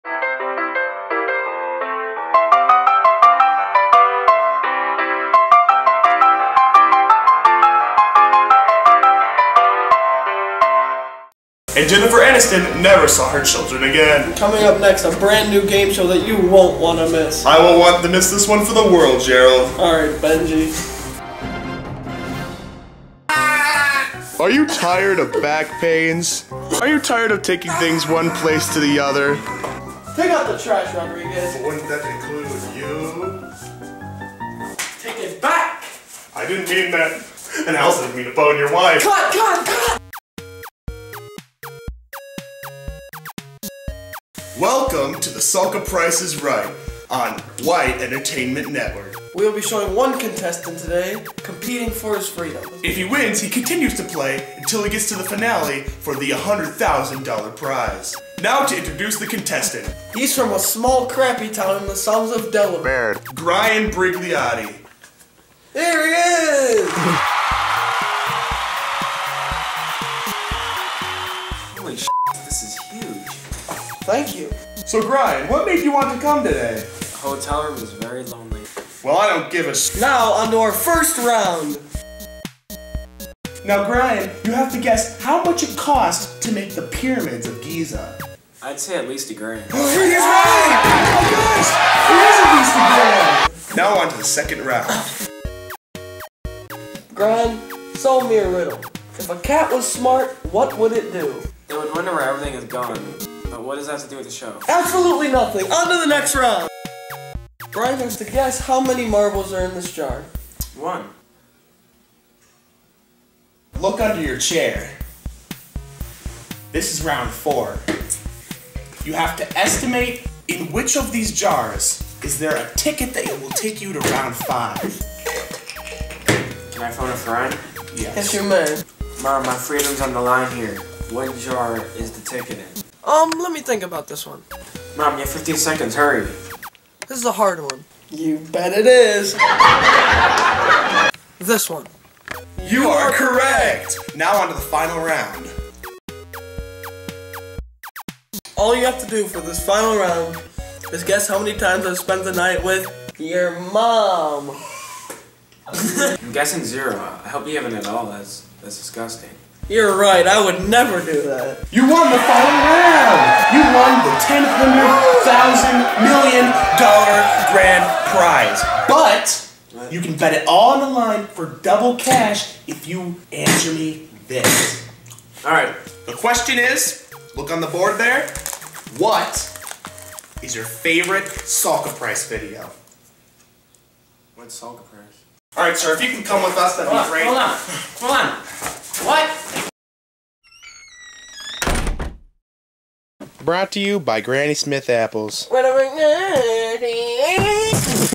Rigged Game Show! - The SulkaPrice is Right
The brand new ORIGINAL game show that will leave you in stiches. This week's contestant is Brian Gigliotti! Welcome him with an open embrace everyone!